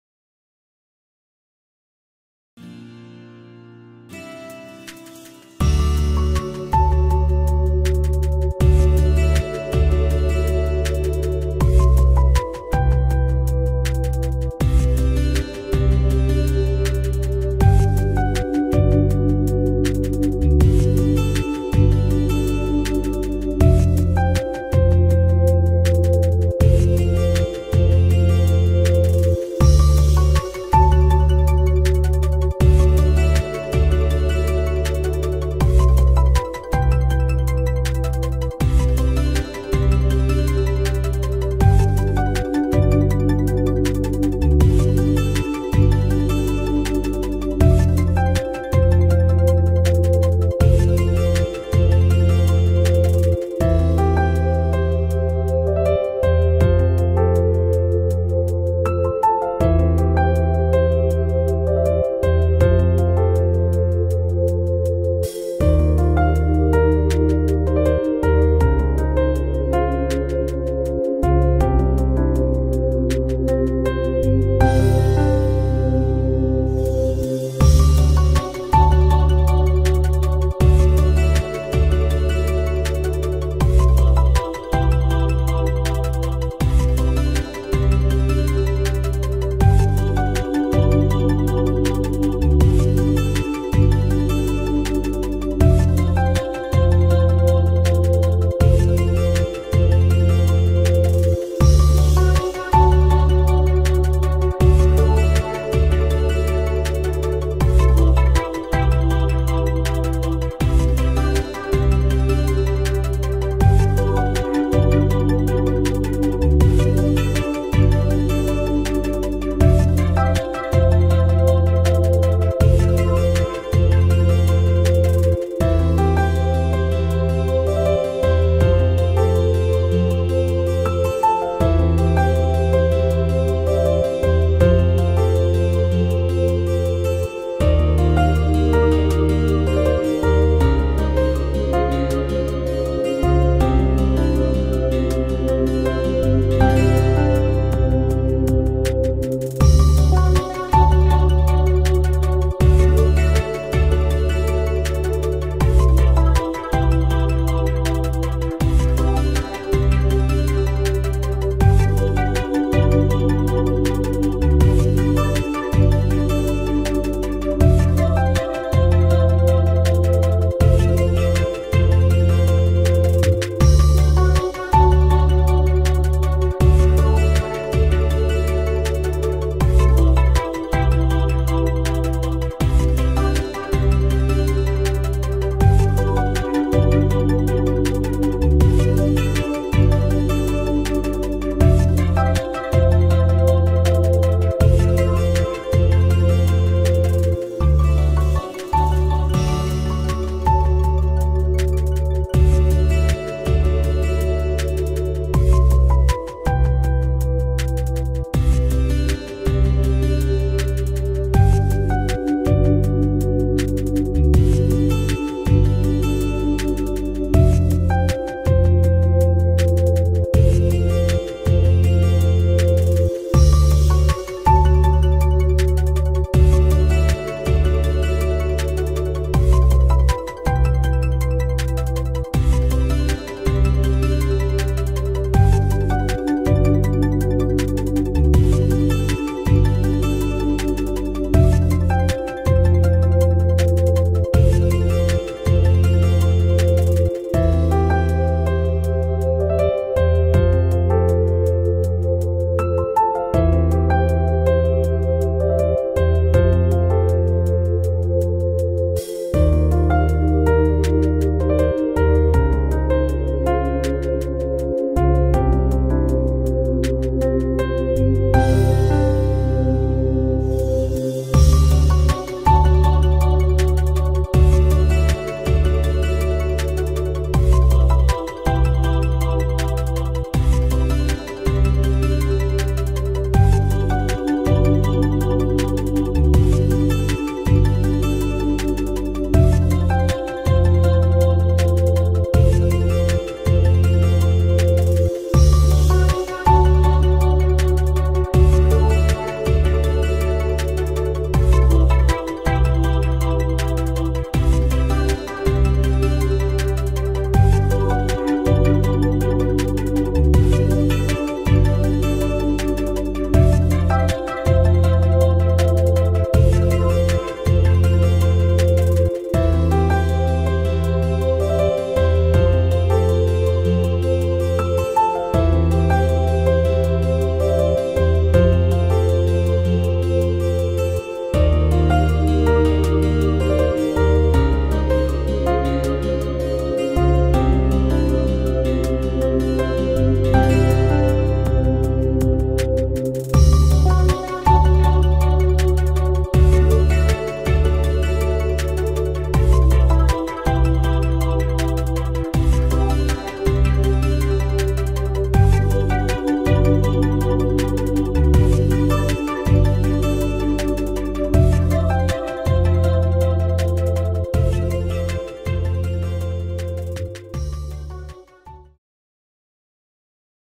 7 Bedroom House for sale in Gauteng | East Rand | Brakpan | Larrendale | 13 Apex Road
7 Bedroom House in Larrendale, 13 apex road, Stunning property in Larrendale!
Spacious family home situated in very secure area, lovely 4 bedroom